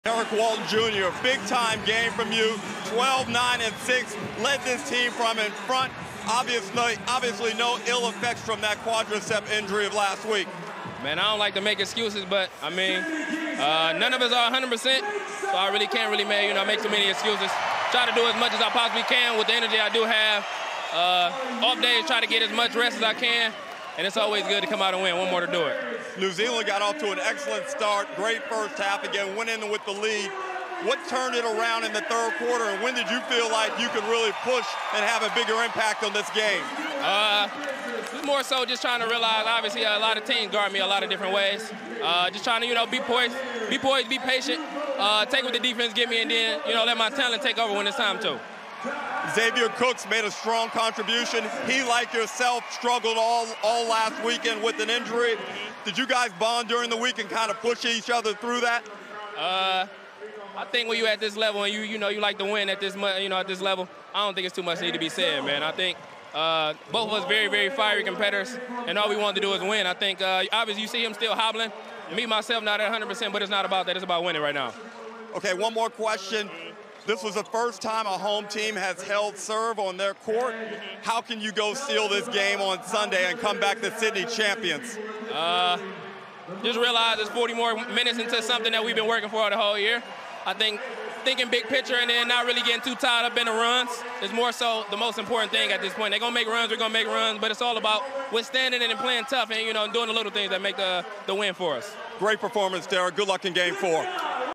Post-game interview with Derrick Walton Jr
NBL commentator Derek Rucker chats to Sydney Kings guard Derrick Walton Jr after his side's game three win against New Zealand at Qudos Bank Arena on March 10.